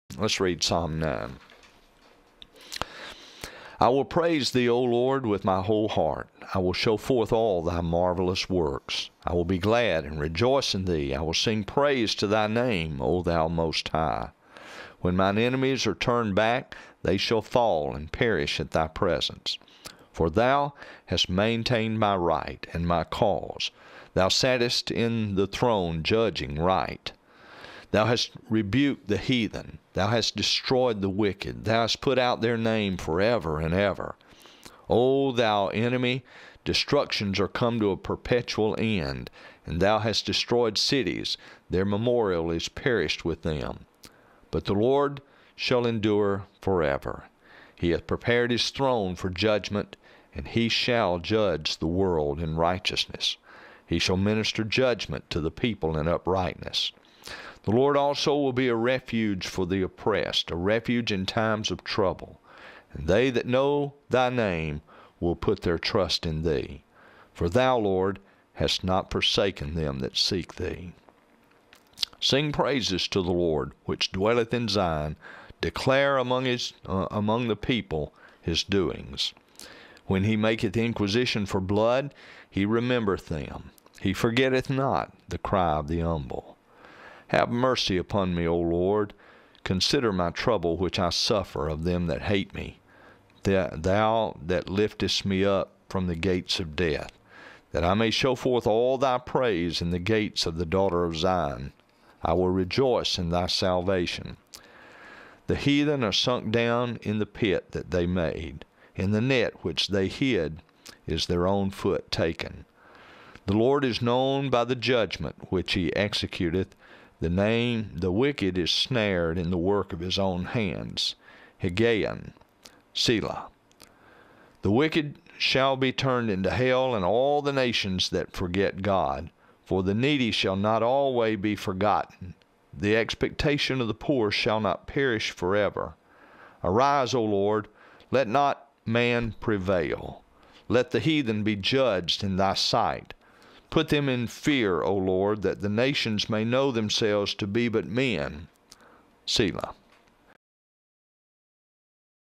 Psalm Chapter 9
King James Bible Reading of Psalm Chapter 9.